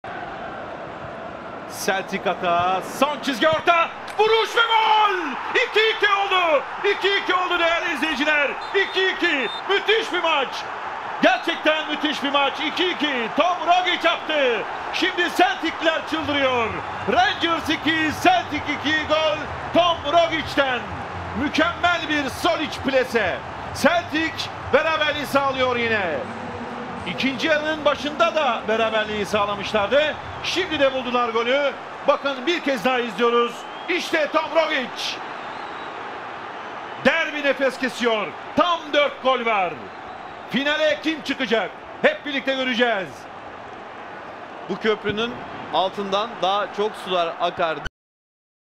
2016 (April 17) Glasgow Rangers - Celtic (Scottish FA Cup) Semifinal Celtic Goal
2016 (April 17) Glasgow Rangers - Celtic (Scottish FA Cup) Semifinal (İskoçya kupası yarı final)